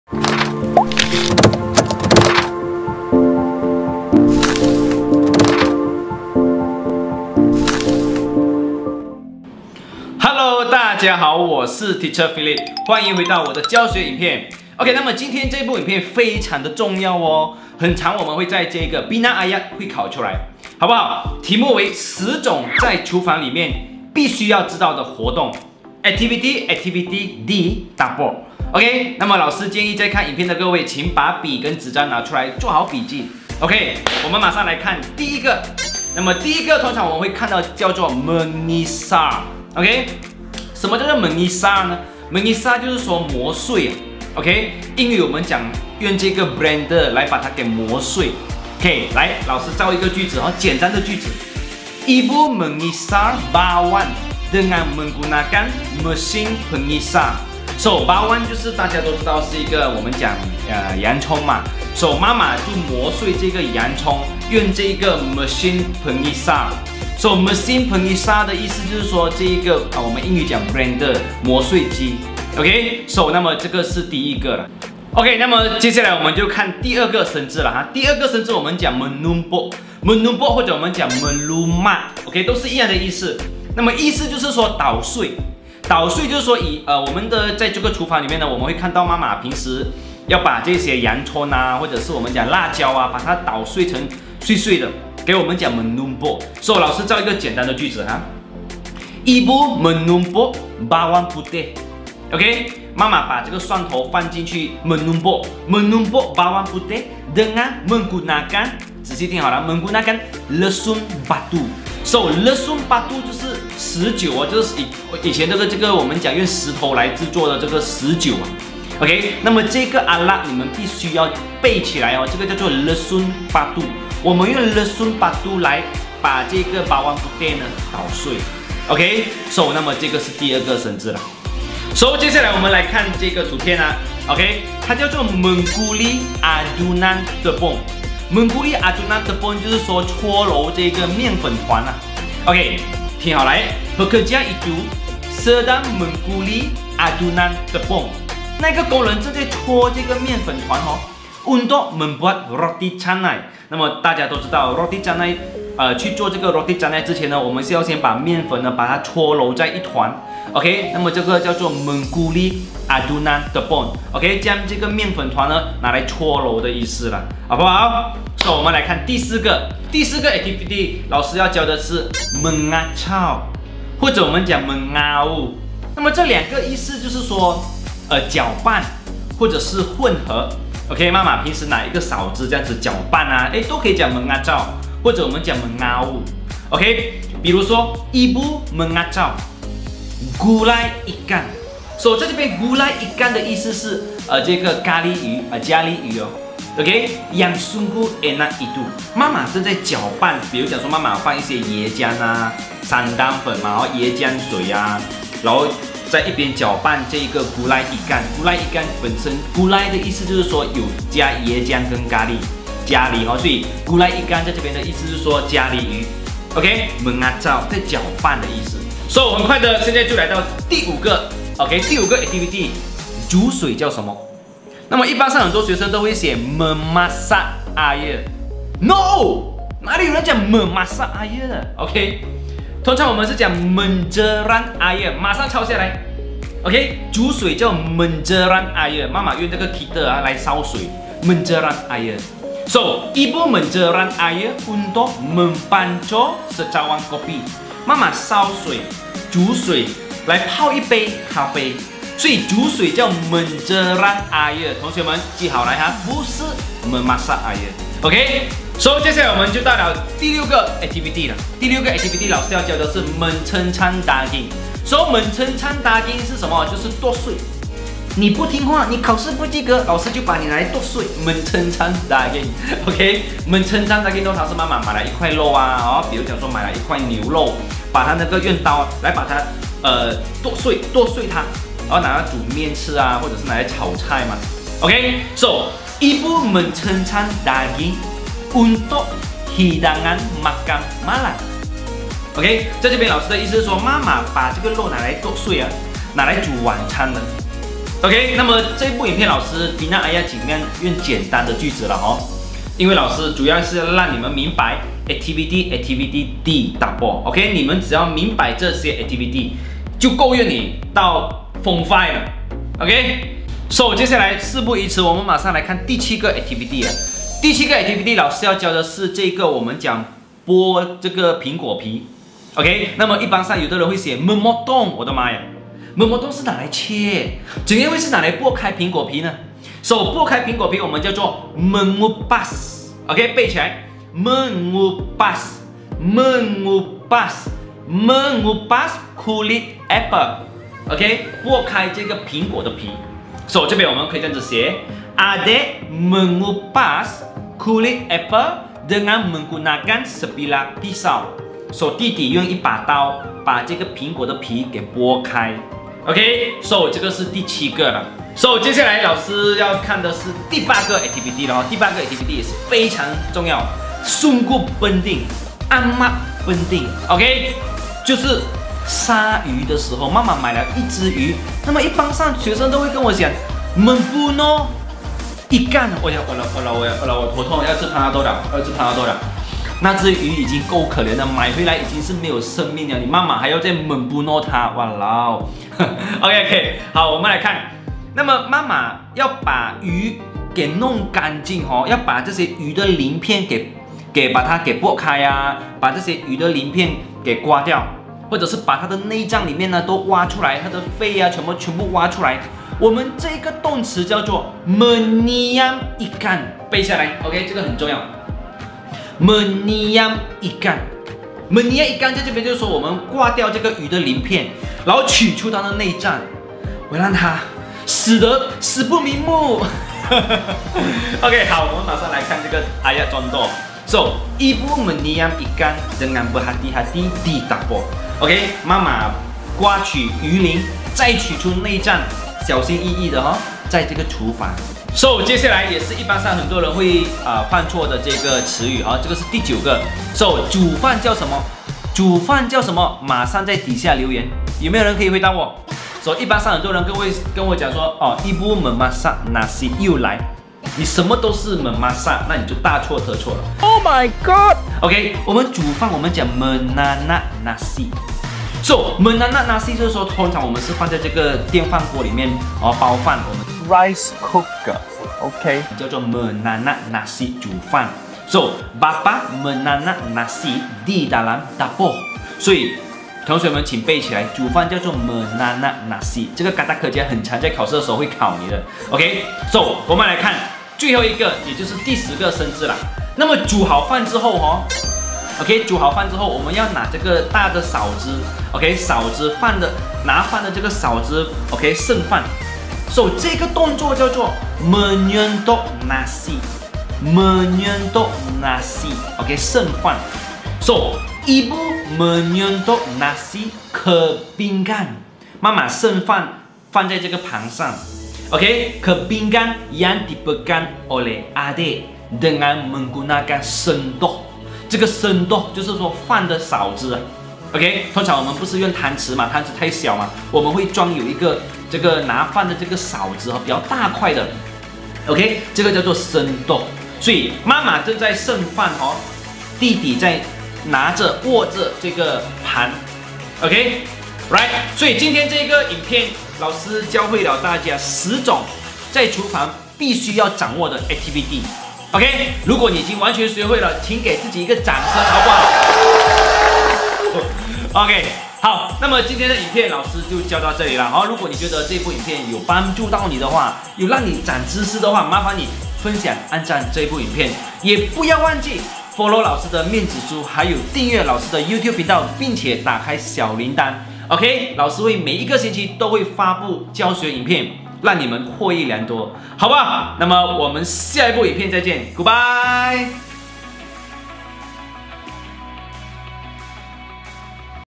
10种在厨房里的活动【正确动词】【Uncle Roger吐槽Teacher Philip教国语！】
煮饭 是 【memasak nasi】 ？
煮水 是 【memasak air】 ？
弄净鱼只 是 【membunuh ikan】？

你们再持续这样幽默地写出这些错误的动词，【HaiYa】 Teacher Philip要吃panadol了 【HaiLat】

快去看【 10个一定要学会的Aktiviti Di Dapur【正确动词】】🤞🤞🤞

学会了这些 ‘热门’ 的【正确动词】， 肯定可以帮助你在考试中得分。

加油孩子们👍👍✌✌😊😊